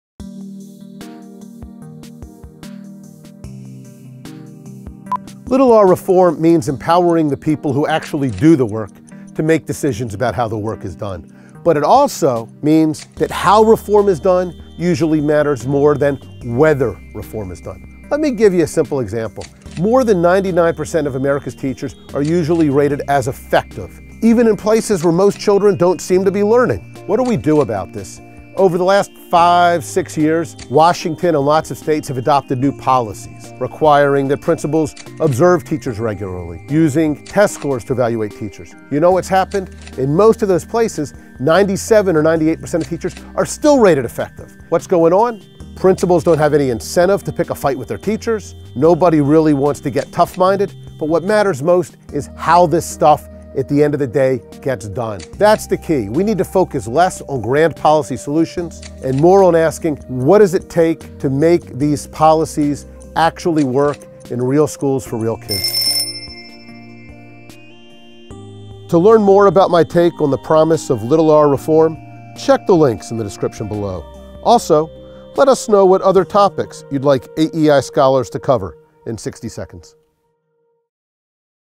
The difference between big and little 'r' education reform | IN 60 SECONDS
Many schools are not performing well, but almost all teachers are rated effective. What gives? AEI's Rick Hess explains that the answer lies with how reforms are done.